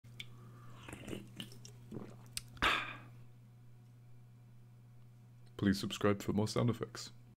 Drinking from a can - Sound Effect
Drinking out of a can sound effects. Use this sound effect without seeking permission from me or anyone else. It's free to use, forever.